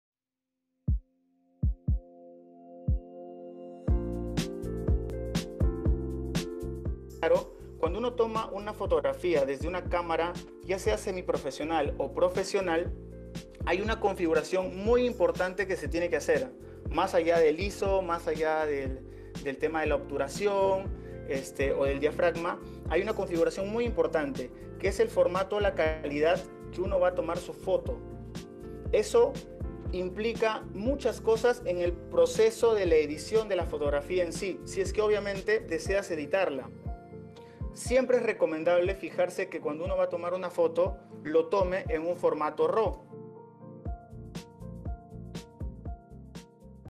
La magia del retoque digital | Evento: La publicidad desde otro ángulo
¿Qué debemos tomar en cuenta antes de realizar una fotografía? 📸

Robert Barbarán, realizador audiovisual 🎥  nos lo cuenta en su charla "La magia del retoque digital"🔮 que formó parta de nuestro evento La Publicidad desde otro ángulo.

Realizado por alumnos de nuestra carrera👏🏼  ¡Disfrútenlo!

Carrera de Comunicación y Publicidad Transmedia📢